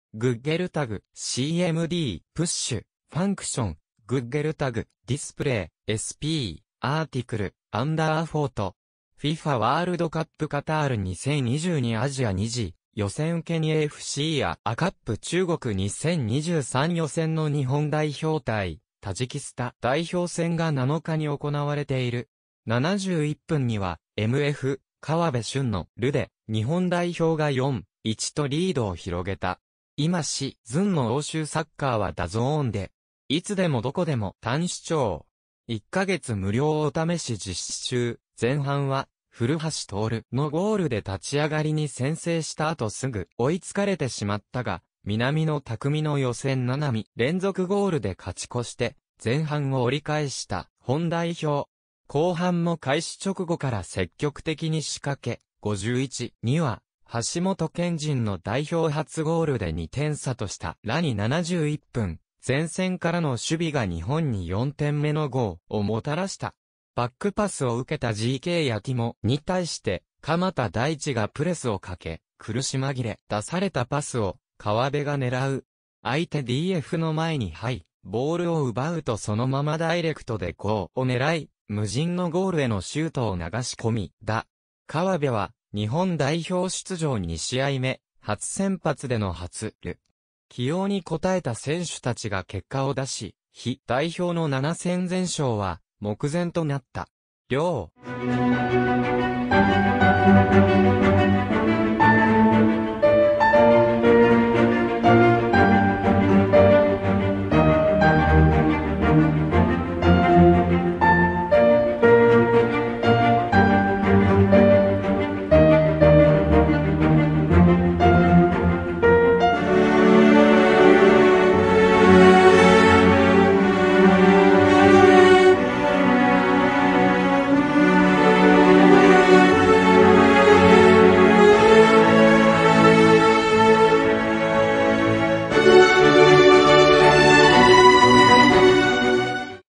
川辺駿も初ゴール！日本代表、前線からの積極的な守備が4点目生み出す
googletag.cmd.push(function() { googletag.display('sp_article_underphoto'); }); 　FIFAワールドカップカタール2022アジア2次予選兼AFCアジアカップ中国2023予選の日本代表対タジキスタン代表戦が7日に行われている。71分にはMF川辺駿のゴールで日本代表が4-1とリードを広げた。 【今シーズンの欧州サッカーはDAZNで！ いつでもどこでも簡単視聴。1ヶ月無料お試し実施中】 　前半は古橋亨梧のゴールで立ち上がりに先制したあとすぐに追いつかれてしまったが、南野拓実の予選7試合連続ゴールで勝ち越して前半を折り返した日本代表。後半も開始直後から積極的に仕掛け、51分には橋本拳人の代表初ゴールで2点差とした。 　さらに71分、前線からの守備が日本に4点目のゴールをもたらした。バックパスを受けたGKヤティモフに対して鎌田大地がプレスをかけ、苦し紛れに出されたパスを川辺が狙う。相手DFの前に入ってボールを奪うとそのままダイレクトでゴールを狙い、無人のゴールへのシュートを流し込んだ。 　川辺は日本代表出場2試合目、初先発での初ゴール。起用に応えた選手たちが結果を出し、日本代表の7戦全勝は目前となった。 【了】